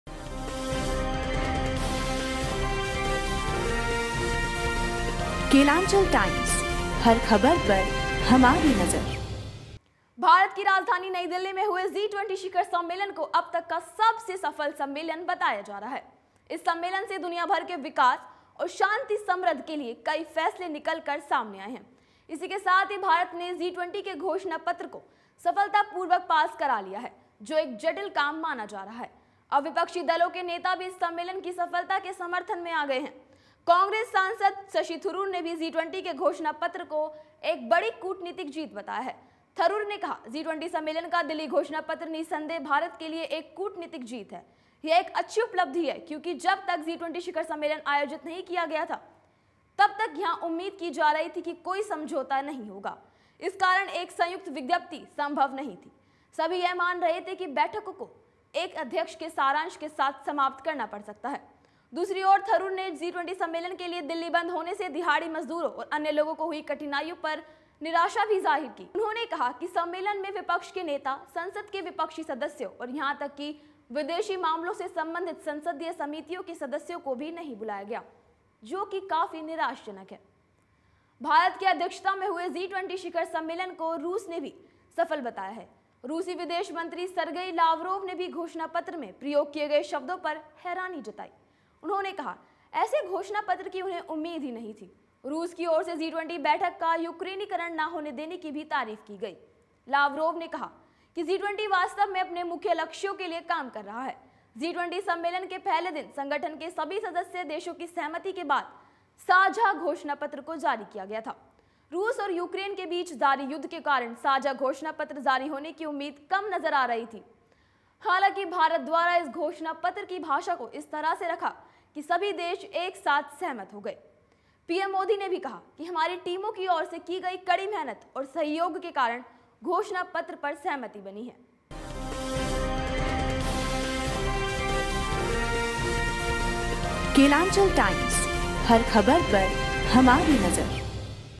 Shashi Tharoor ने जमकर की G20 डिक्लेरेशन की तारीफ
भारत की राजधानी नई दिल्ली में हुए जी20 शिखर सम्मेलन को अब तक का सबसे सफल सम्मेलन बताया जा रहा है। इस सम्मेलन से दुनियाभर के विकास और शांति-समृद्धि के लिए कई फैसले निकलकर सामने आए हैं।